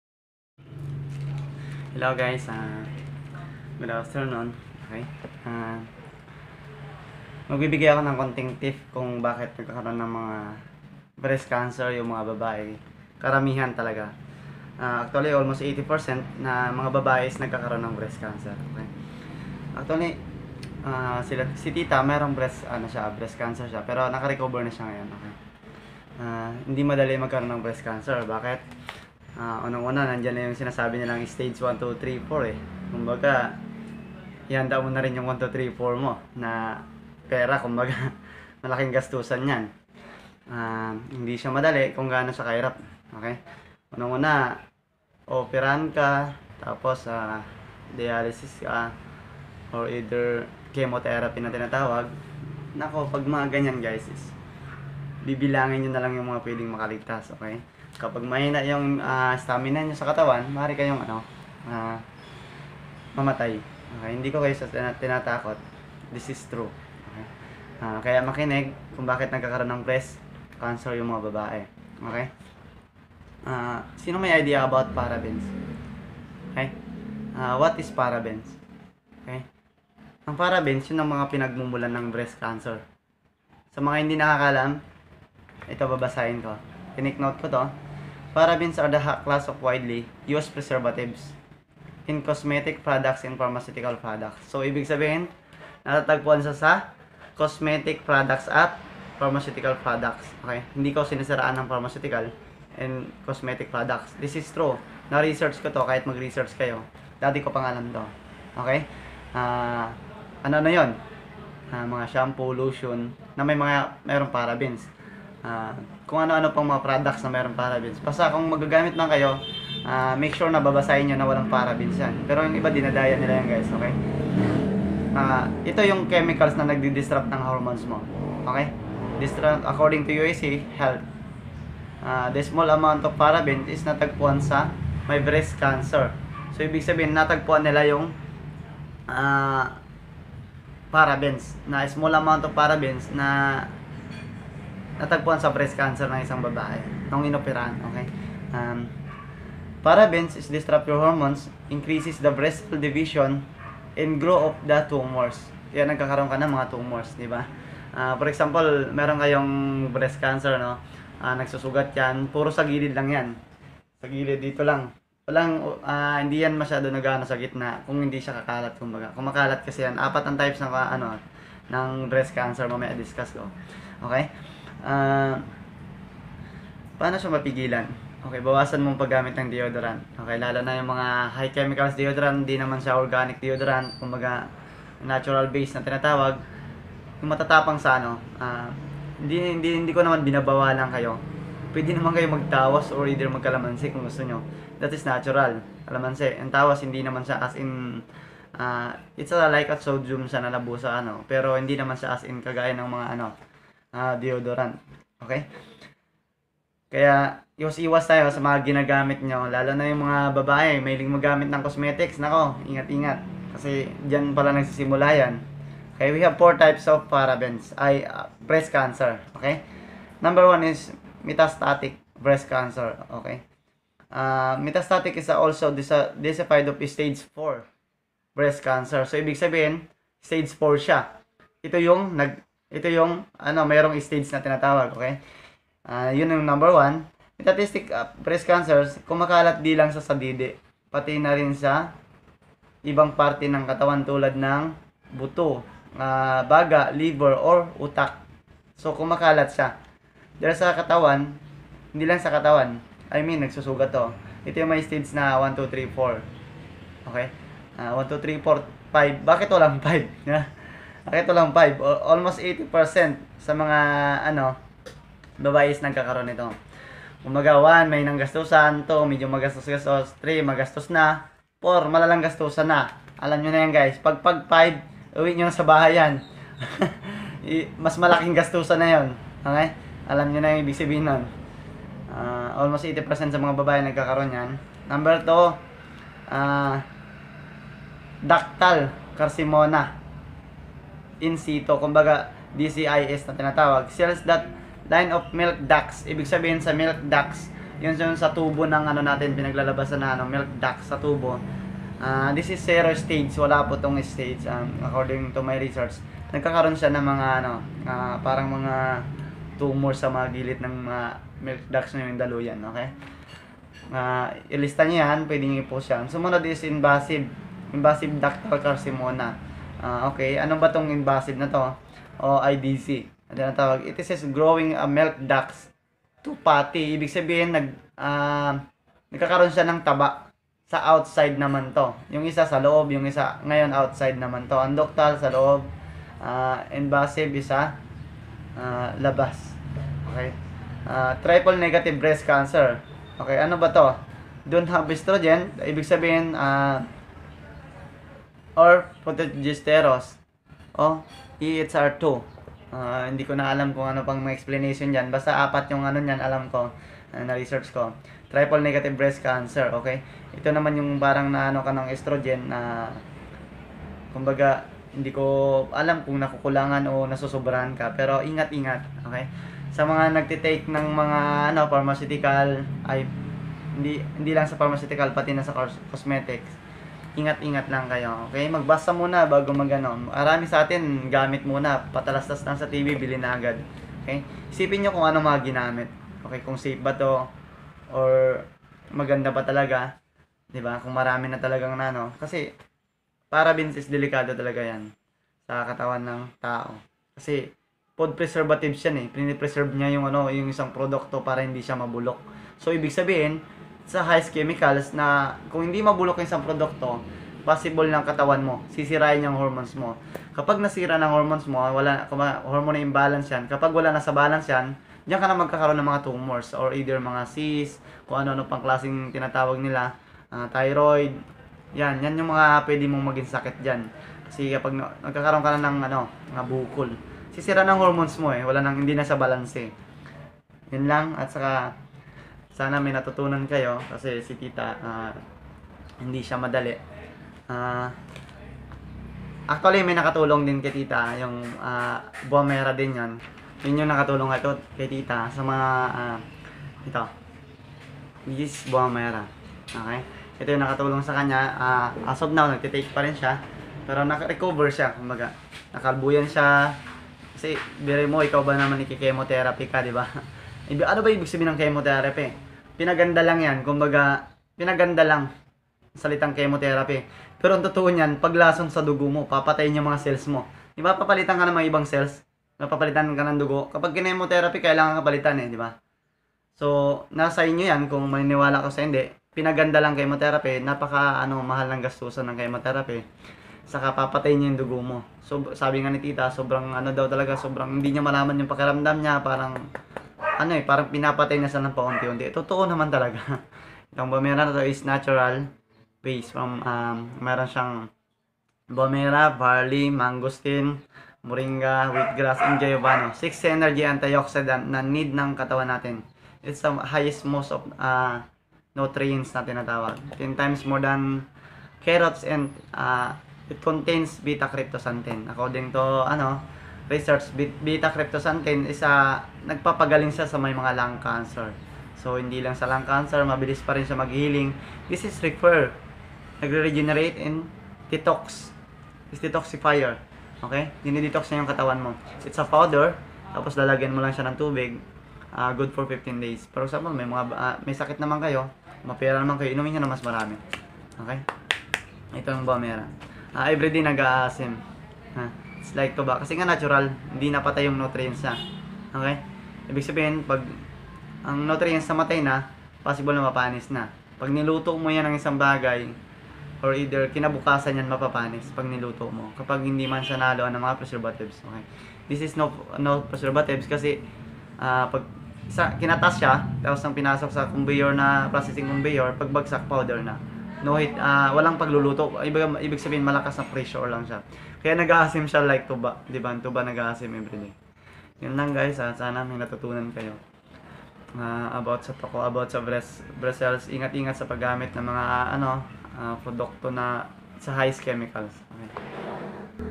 Bakit nagkakaroon ng Breast cancer ang mga babae.
There are 4 types of breast cancer.